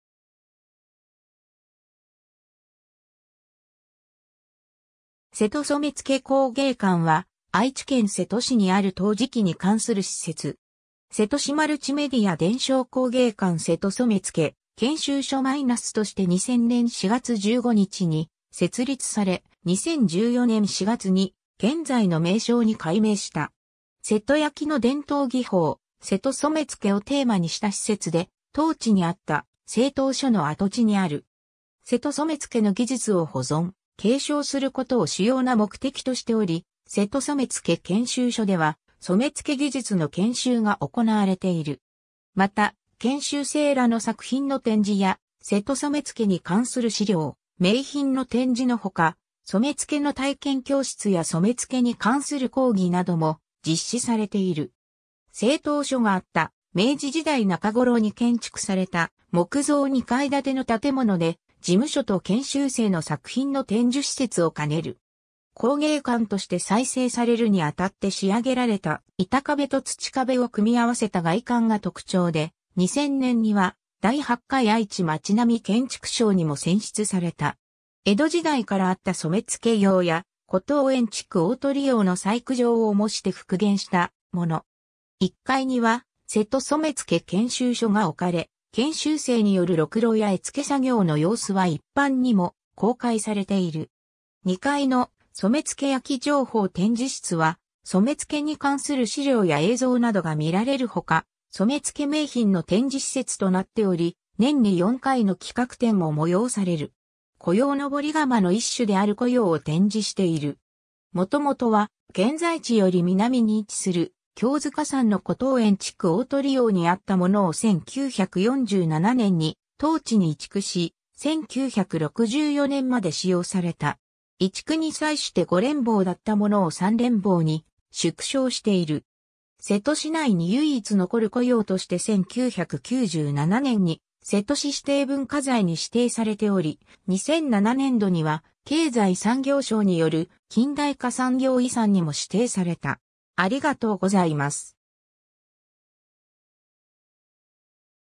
瀬戸染付工芸館
瀬戸染付工芸館（せとそめつけこうげいかん）は愛知県瀬戸市にある陶磁器に関する施設。
 瀬戸市マルチメディア伝承工芸館 -瀬戸染付研修所-として2000年（平成12年）4月15日に設立され、2014年（平成26年）4月に現在の名称に改名した。
瀬戸焼の伝統技法「瀬戸染付」をテーマにした施設で、当地にあった製陶所の跡地にある。
瀬戸染付の技術を保存・継承することを主要な目的としており、「瀬戸染付研修所」では染付技術の研修が行なわれている。
また、研修生らの作品の展示や瀬戸染付に関する資料・名品の展示のほか、染付の体験教室や染付に関する講義なども実施されている。
 製陶所があった明治時代中頃に建築された木造2階建ての建物で、事務所と研修生の作品の展示施設を兼ねる。
工芸館として再生されるにあたって仕上げられた板壁と土壁を組み合わせた外観が特徴で、2000年（平成12年）には第8回愛知まちなみ建築賞にも選出された。
 江戸時代からあった染付窯屋「古陶園竹鳳窯」（伊藤伊平家）の細工場を模して復元したもの。
1階には「瀬戸染付研修所」が置かれ、研修生によるろくろや絵付け作業の様子は一般にも公開されている。
2階の「染付焼情報展示室」は染付に関する資料や映像などが見られるほか、染付名品の展示施設となっており、年に4回の企画展も催される。
 古窯 登り窯の一種である古窯（こがま）を展示している。
元々は現在地より南に位置する経塚山（弘法山）の古陶園竹鳳窯にあったものを1947年（昭和22年）に当地に移築し、1964年（昭和39年）まで使用された。
移築に際して五連房だったものを三連房に縮小している。
瀬戸市内に唯一残る古窯として1997年（平成9年）に瀬戸市指定文化財に指定されており、2007年度には経済産業省による近代化産業遺産にも指定された。